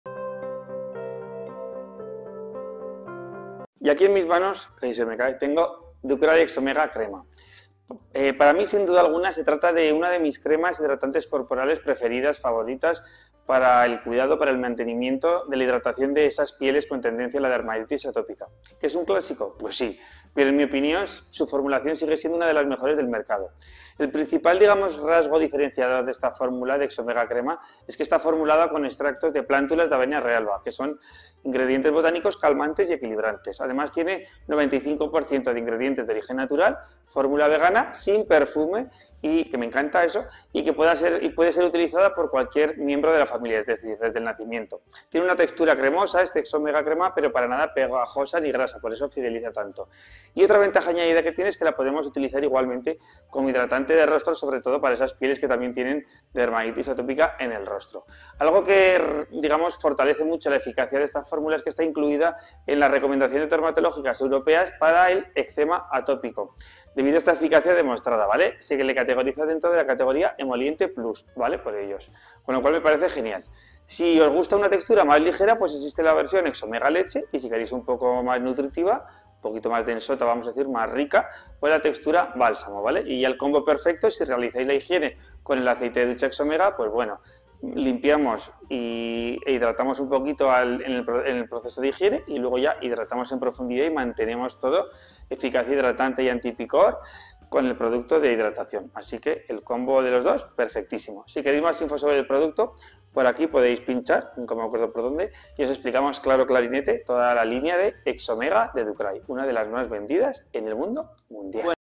ADERMA Exomega Control Crema Emoliente Opiniones 💖 Crema Anti-Rascado A-Derma🌷 FARMACIA SENANTE
Review completa de la crema emoliente ADerma Exomega Control.